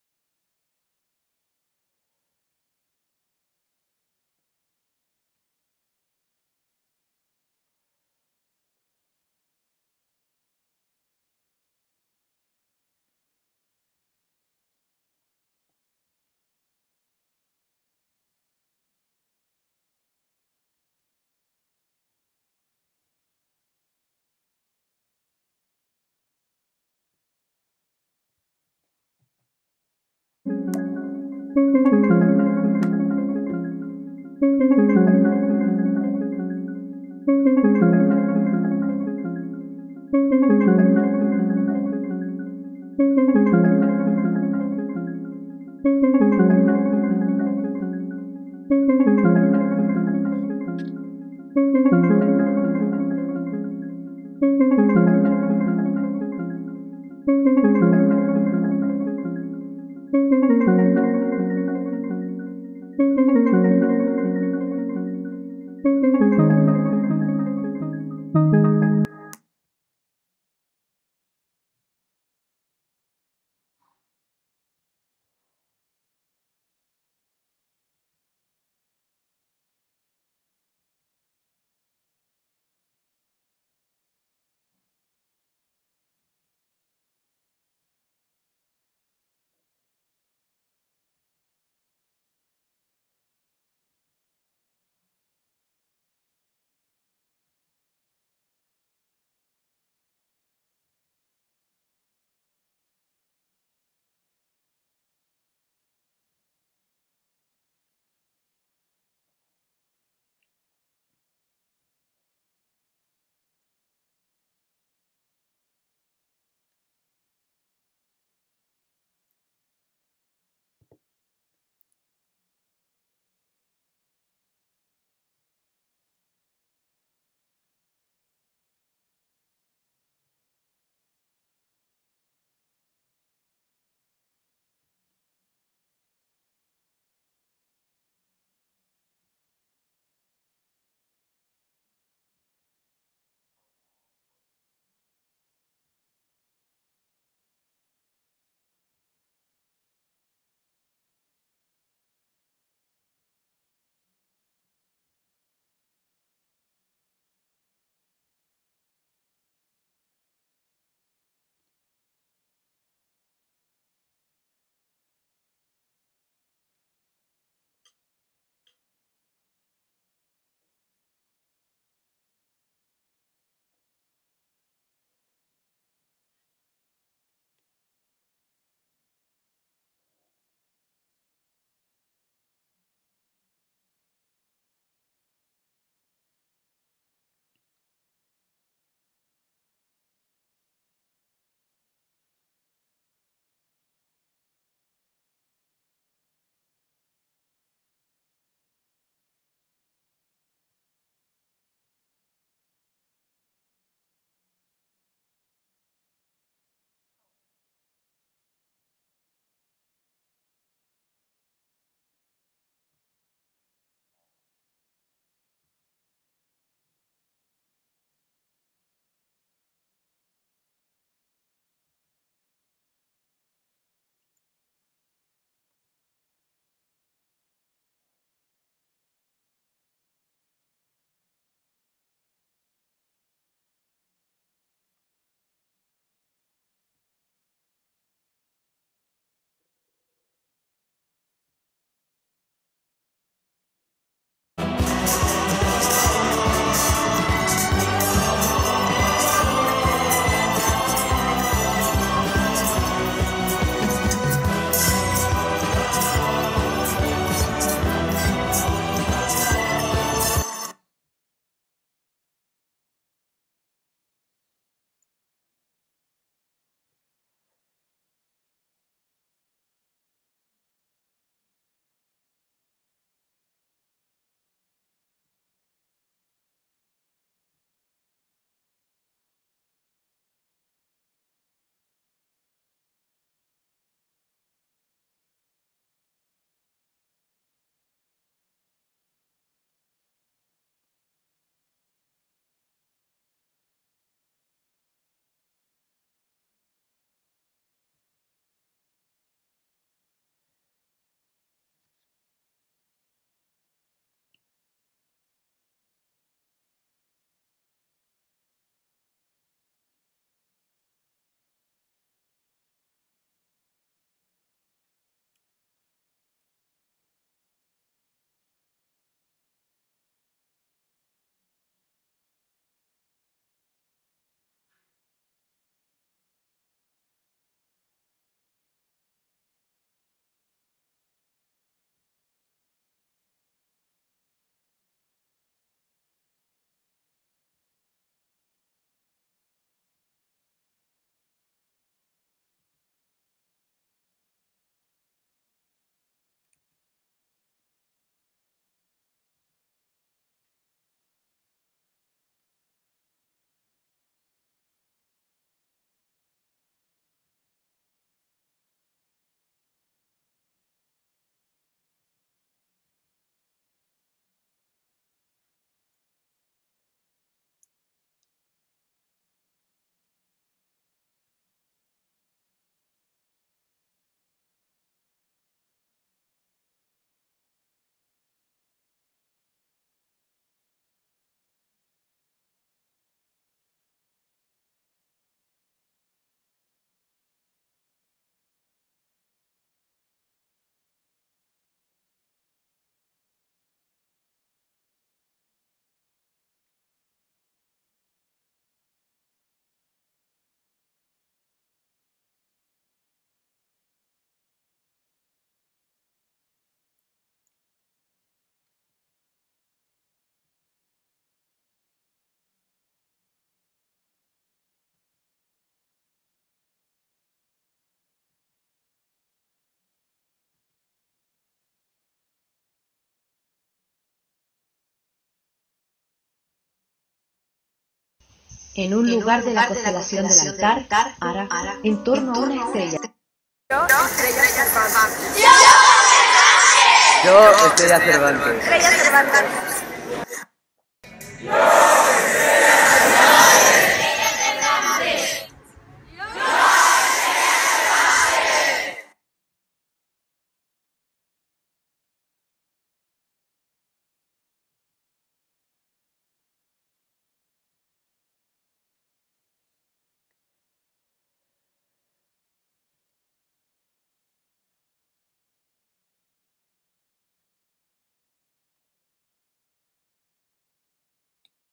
Estrella Cervantes (Vídeo) - Francisco Javier Gorgas
Un vídeo informativo del proyecto de la Estrella Cervantes elaborado por Francisco Javier Gorgas, astrofísico de la UCM y presidente de la Sociedad Española de Astronomía.